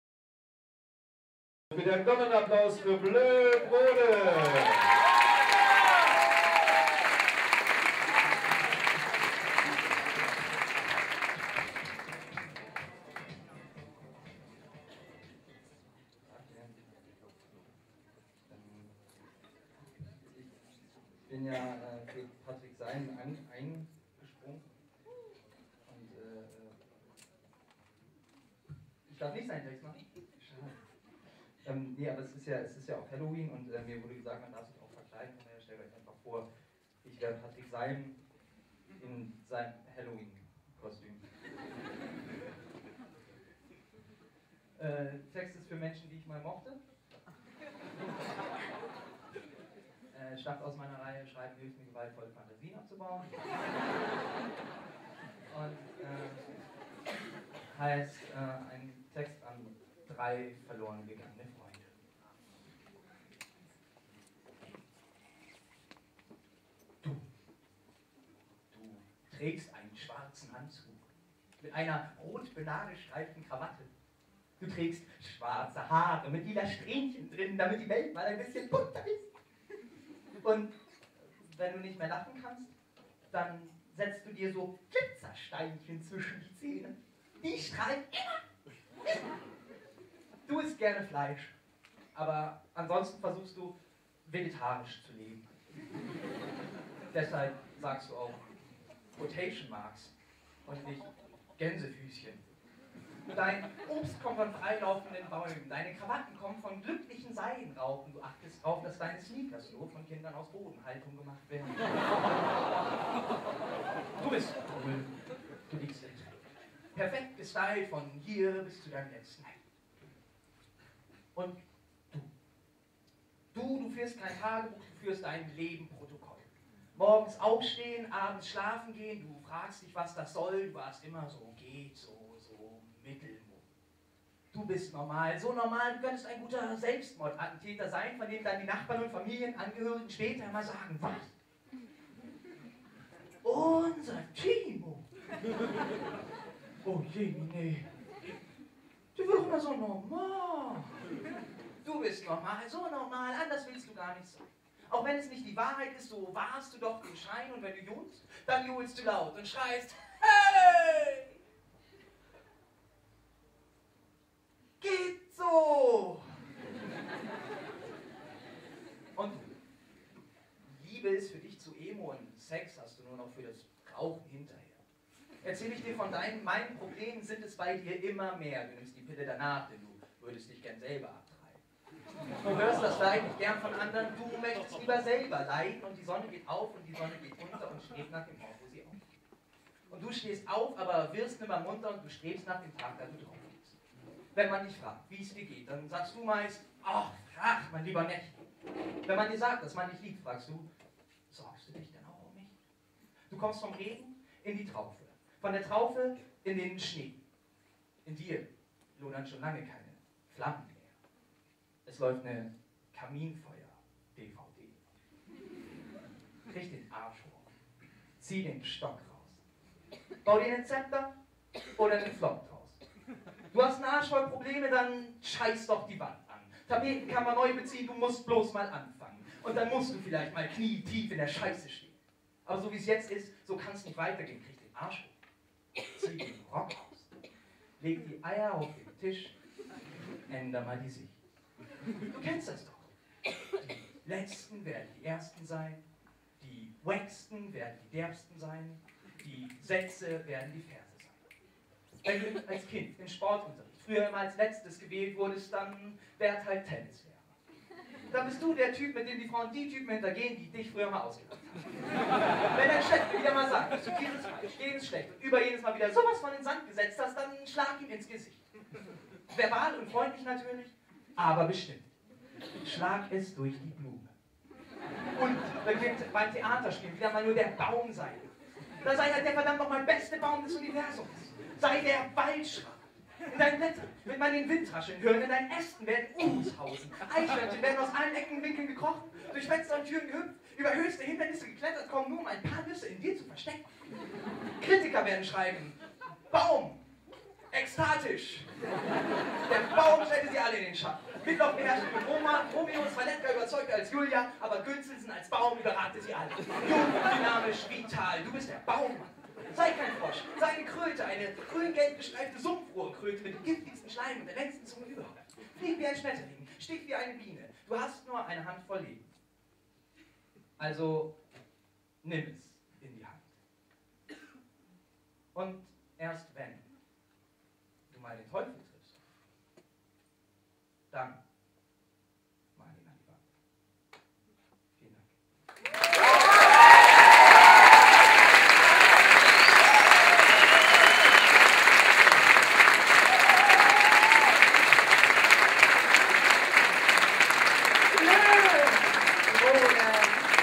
WORD UP! Bleu Brode
31. Oktober 2011, WORD UP! Poetry Slam @ DAI Heidelberg